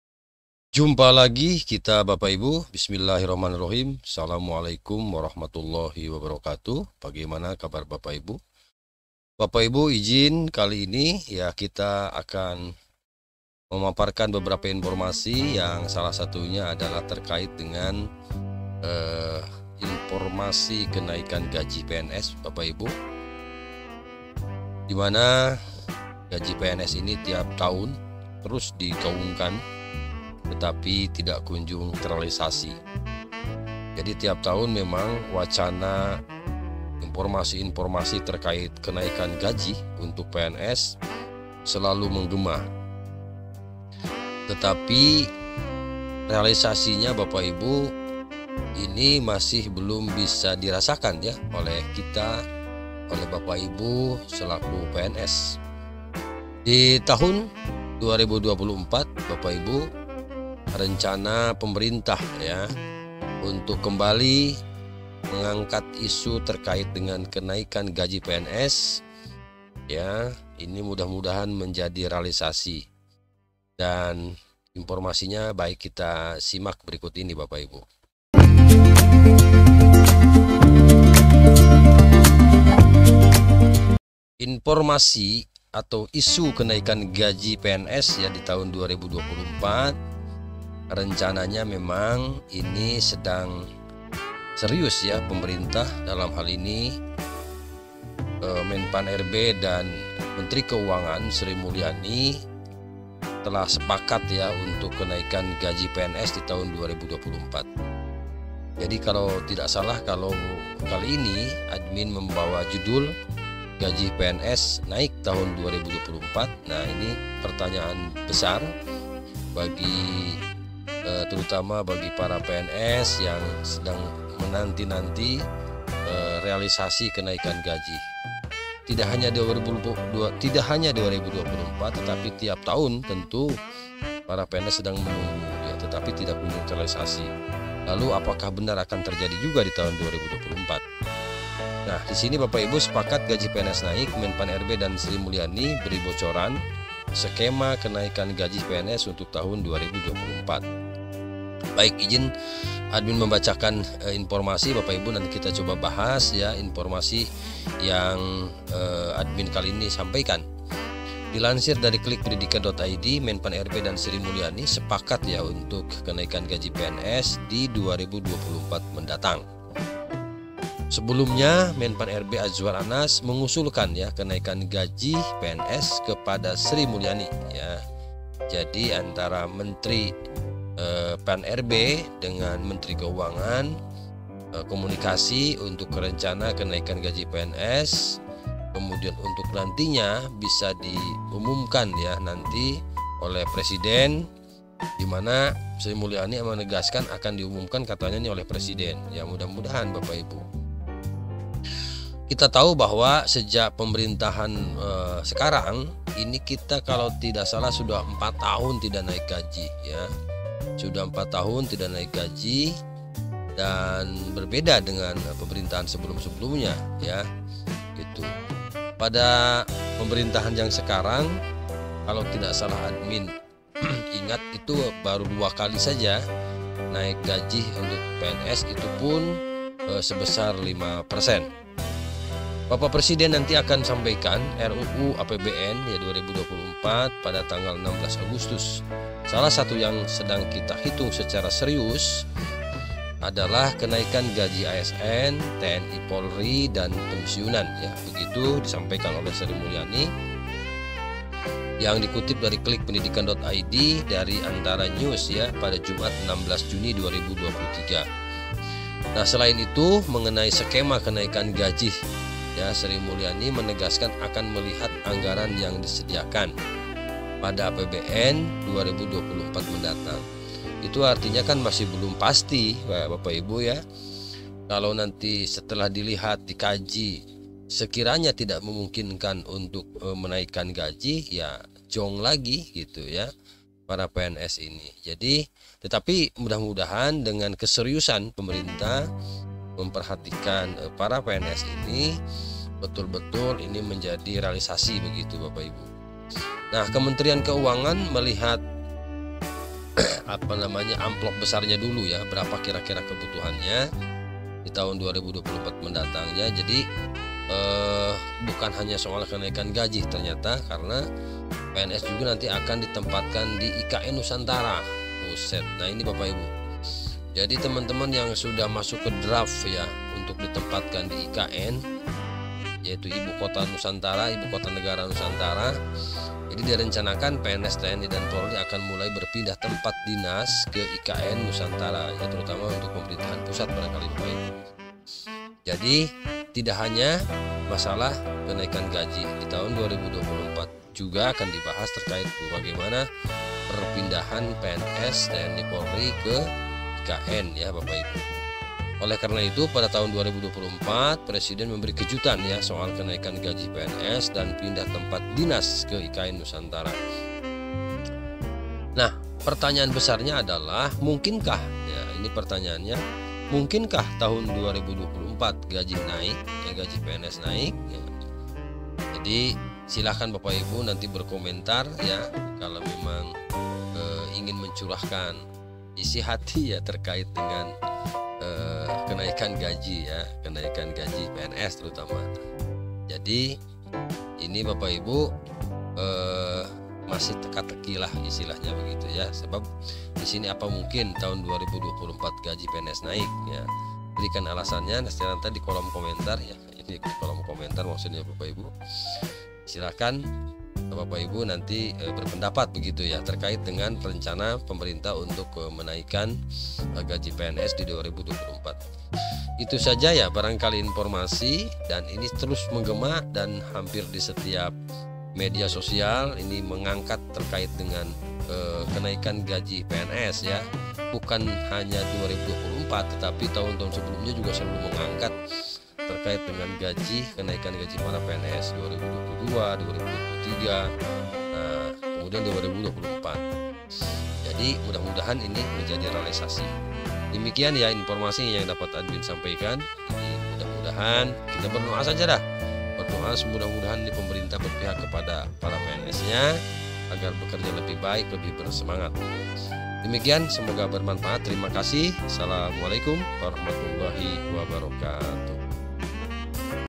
Kenaikan Gaji ASN (PNS/PPPK) Tahun 2024 || Kesepakatan Kemenkeu dan Menpan RB
Terima Kasih telah bergabung bersama channel @kangedibae semoga informasi yang disajikan dapat mengedukasi dan bermanfaat bagi orang banyak. Terima kasih.